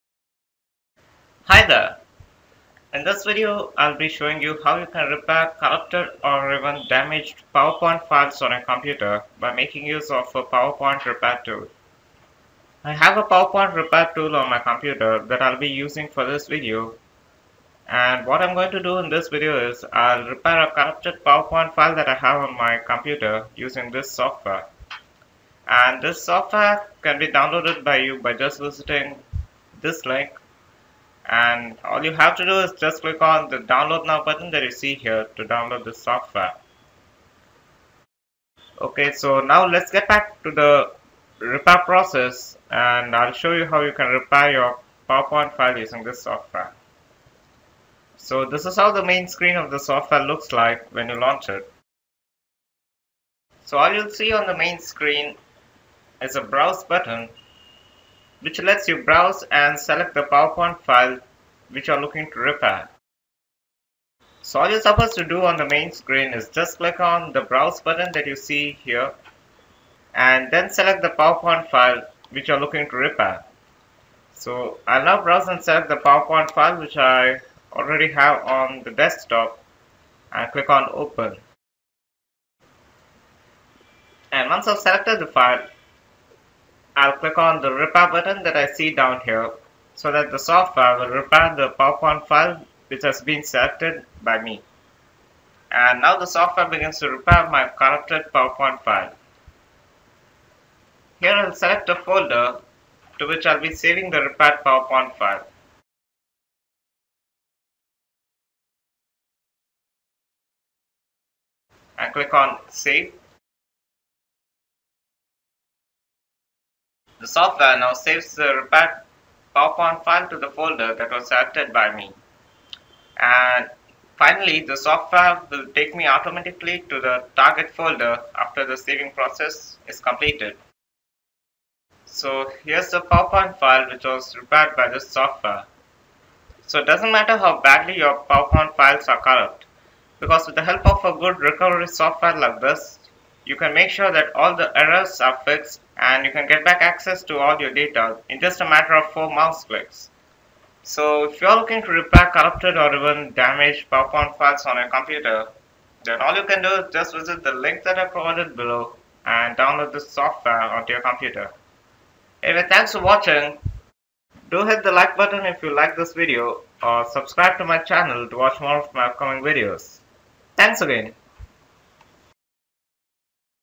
Powerpoint File Repair Tool to Fix Corrupt Damaged PPT Files
Looking for a tool to repair your corrupted or damaged PPT files, then this PPT file repair tool will solve all your problems and finally able to fix your PPT files in a matter of seconds. Interested to learn how? Watch the video to find it out. The tool we are going to use is Remo Repair PowerPoint which eventually repairs all your PPT PowerPoint files using its world-class algorithm.

You should follow the below steps in order to fix ‘PowerPoint file is not opening’ error. There are many scenarios which causes PowerPoint PPT files to be corrupt and in order to fix this, you need this Remo Repair PPT tool. Download the software from the above link and install it on your computer. This PowerPoint repair tool is designed especially for Windows and hence you can use this tool on all Windows operating systems such as Windows vista, Windows 7, Windows 8 and Windows 10. It supports both 32-bit and 64-bit versions of Windows OS so you won’t get any compatibility issues while installing this PowerPoint repair tool.

Corrupt PowerPoint file repair is one of the main issues users are facing especially when they are getting ready for a presentation. They should have prepared all the contents in the slides of a PPT file and what it is like when they find it broken or corrupted or damaged.

Remo Repair is the perfect repair tool for PowerPoint files and hence you don’t need to worry to fix your PowerPoint files. Follow the below steps to know how his PPT file repair tool works.
Open Remo Recover software to repair PowerPoint PPT files after successful download.

You will be able to see a screen now with a box where you should load your corrupt PPT or PPS or PPTX file to repair.
Click on the ‘Browse’ which lets you to select the damaged PowerPoint file to repair.

Then, finally click on ‘Repair’ button to process the PPTX file repair.
The software will now show you the progress screen. Wait for the process to complete.

Now, the PPT file repair tool will show you a box to choose the destination folder where the repaired file is to be saved.
Then the repaired file will be saved to the target folder instantly. All you can do is to just see the PowerPoint PPTX file for all the slides.
Of course, you will find all the slides with headers and footers with the OLE objects and all the attachments such as videos, pictures, etc.

By opting for full version software license, you will be able to not only preview but also can able to save the repaired PowerPoint files. So, you just repaired your corrupt PPT files using the good repair tool for PowerPoint files.